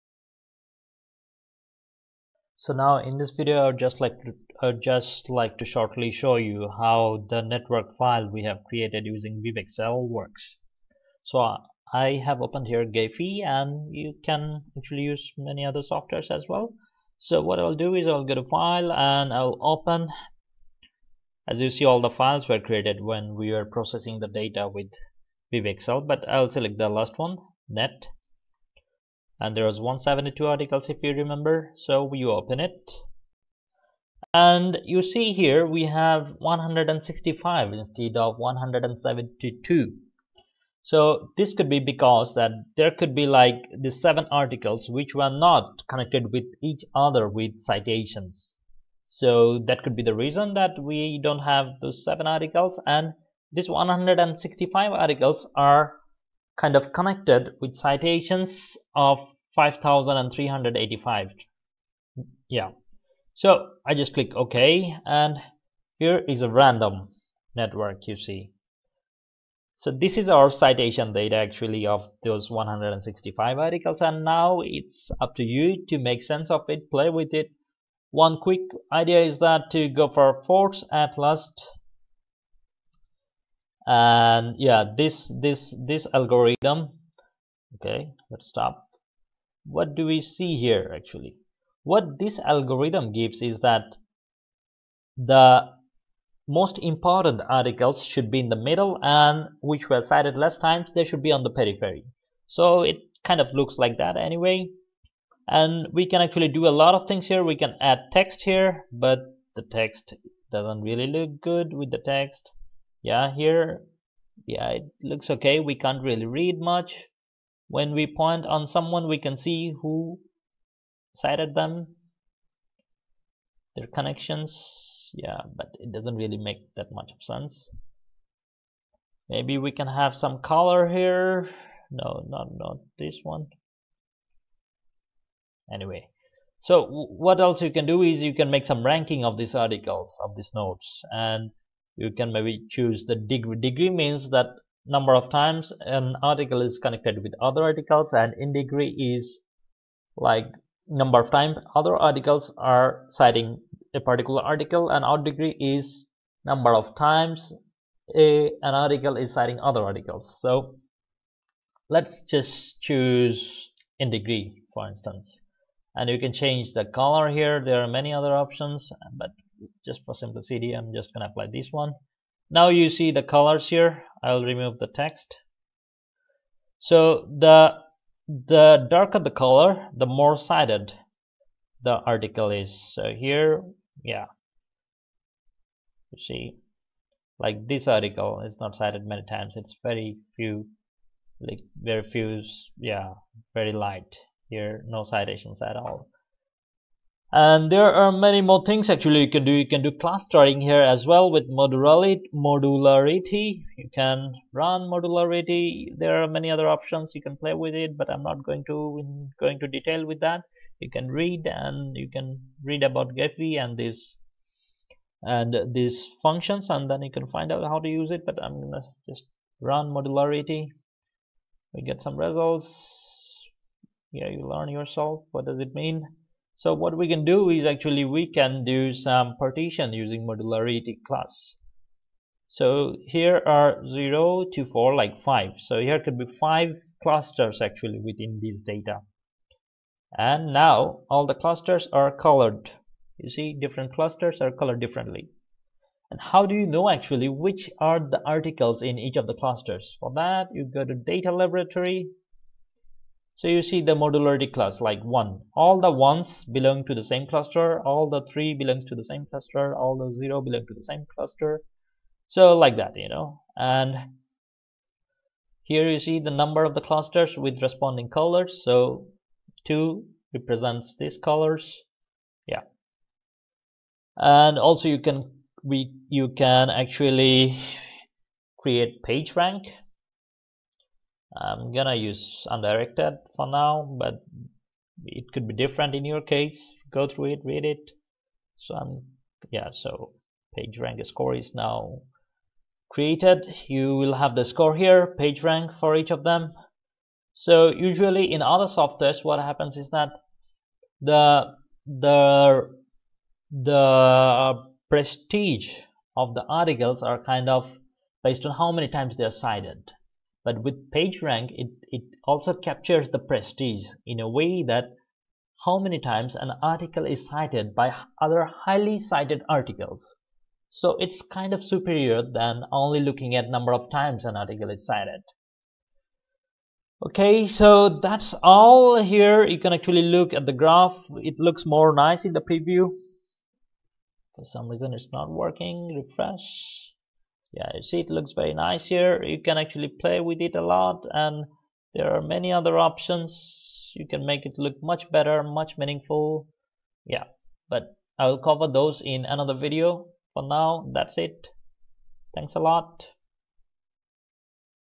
Bibliometrics (9): Citation Network Analysis using Gephi
You need to prepare data to be processed by Gephi.
Note that, data prepared in the video "Bibliometric (8): Preparing a Network File using BibExcel" is used for analysis in this video.